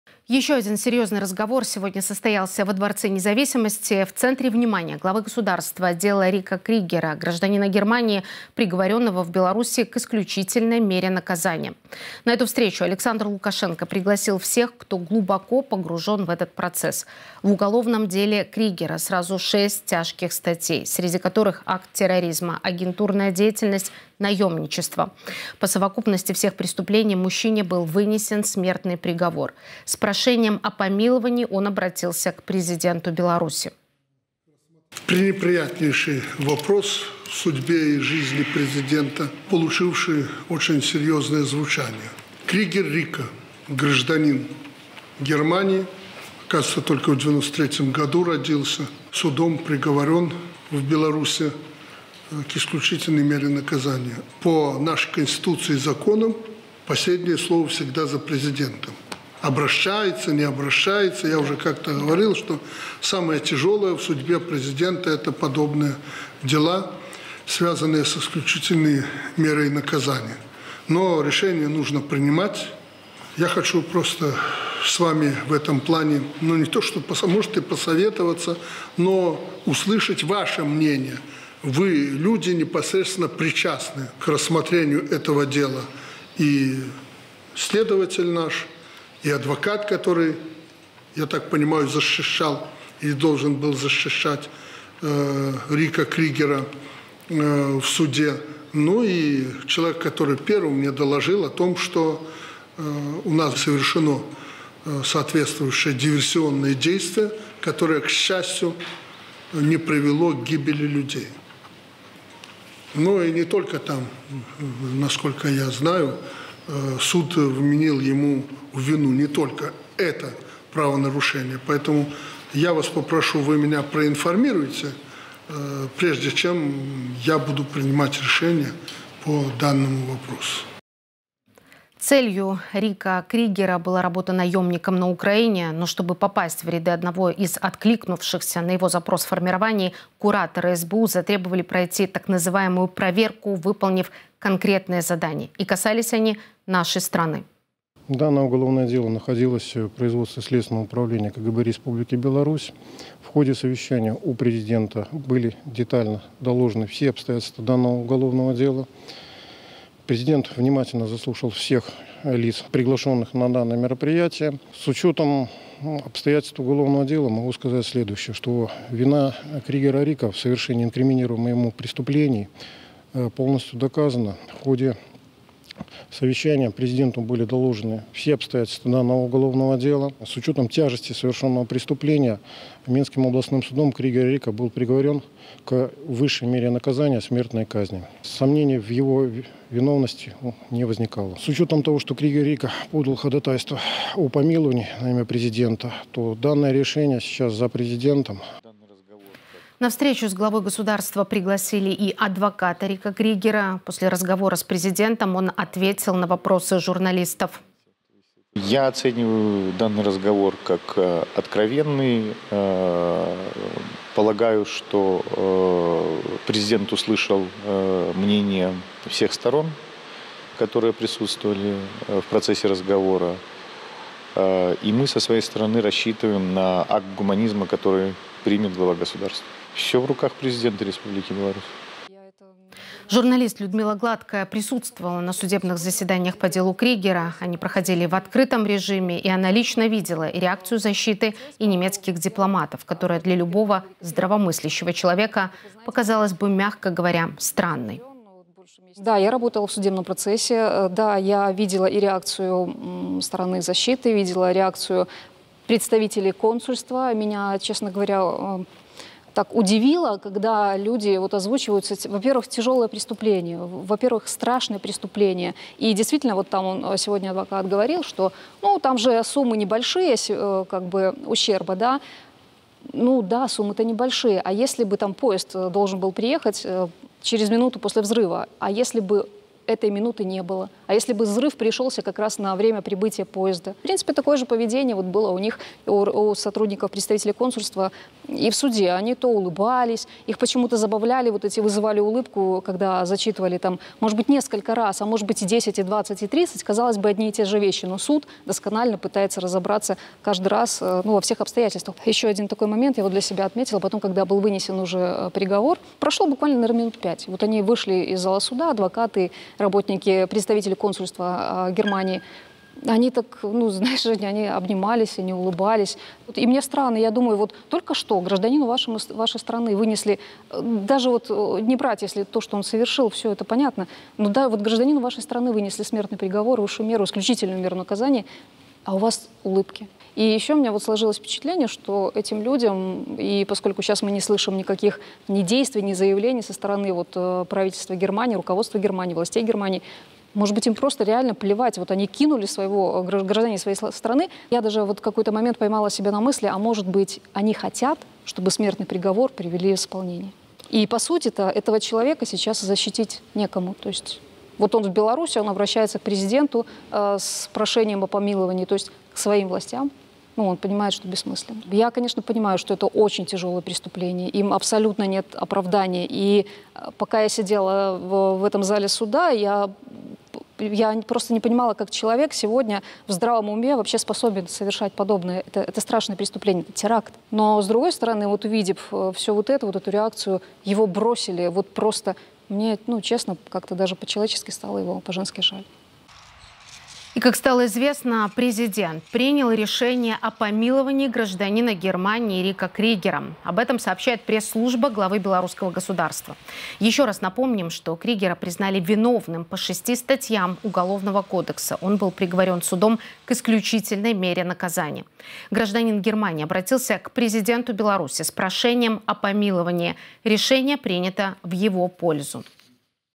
Лукашенко помиловал гражданина Германии Рико Криегера!
Серьёзный разговор 30 июля состоялся во Дворце Независимости. В центре внимания главы государства дело Рико Кригера, гражданина Германии, приговорённого в Беларуси к исключительной мере наказания.